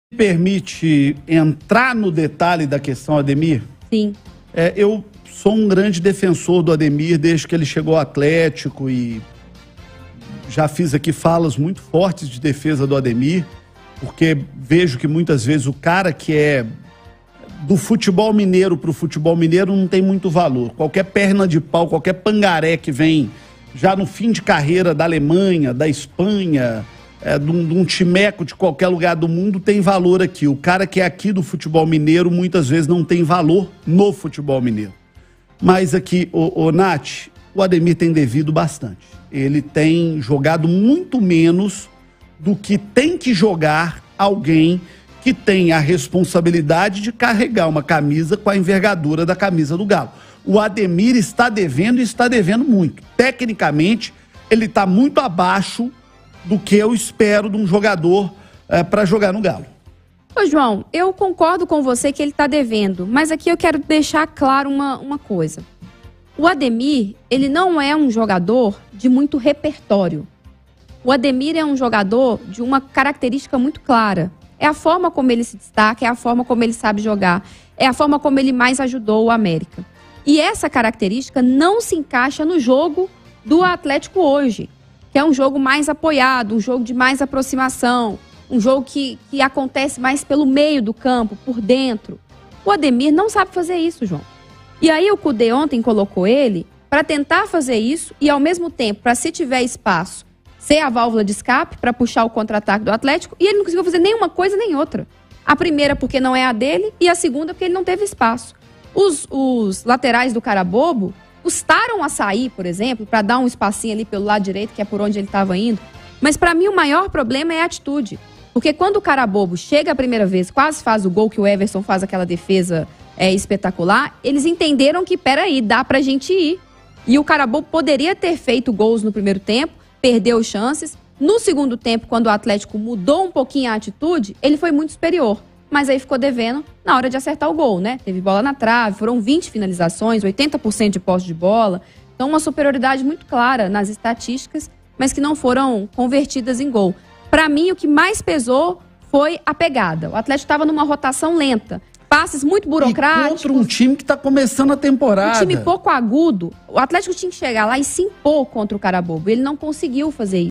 ADEMIR ESTÁ DEIXANDO A DESEJAR NO ATLÉTICO? JOÃO VITOR XAVIER E NATH FIUZA OPINAM
Ademir está abaixo do esperado no Atlético? Ele ainda merece chances no Galo?